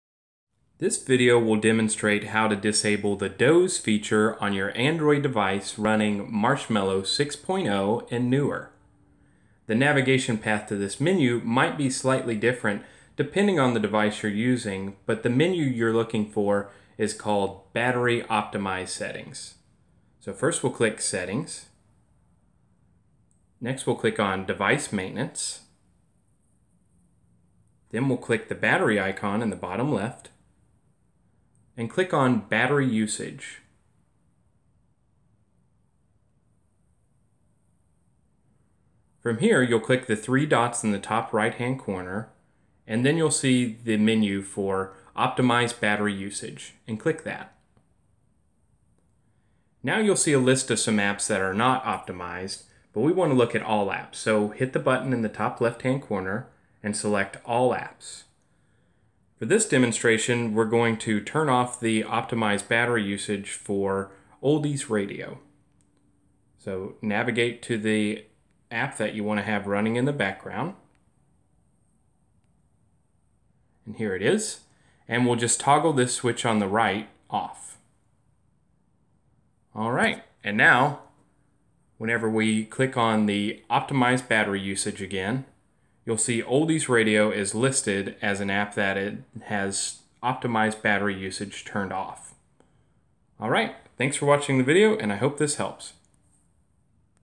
How to disable Doze Settings on Android 6.0 and newer
This video demonstrates how to disable Doze on Android 6.0 and newer